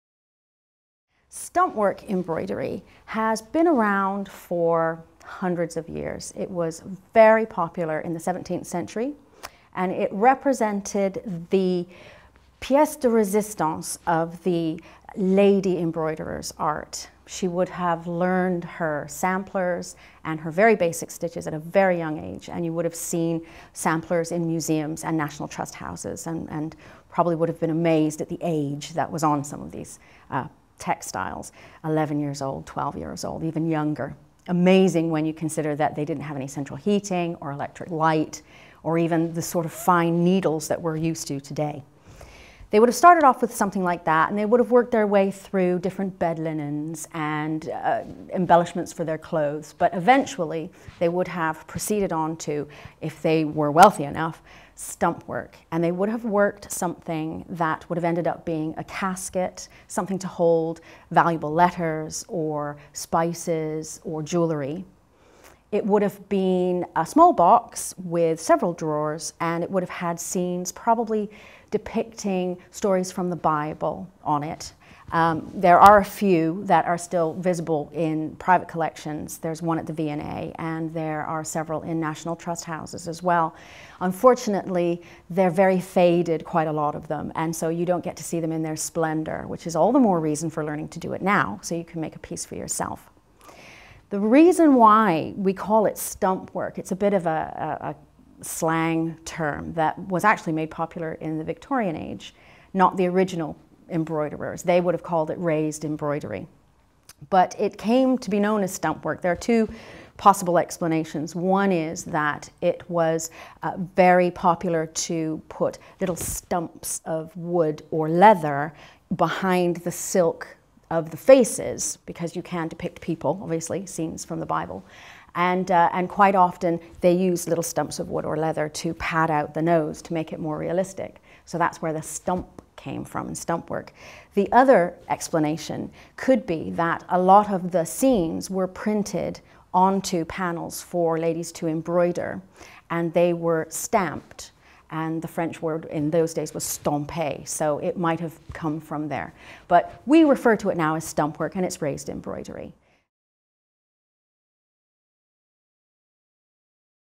History of Stumpwork by Kelley Aldridge
History of Stumpwork  - this is background information on this ancient style of embroidery technique from Kelley Aldridge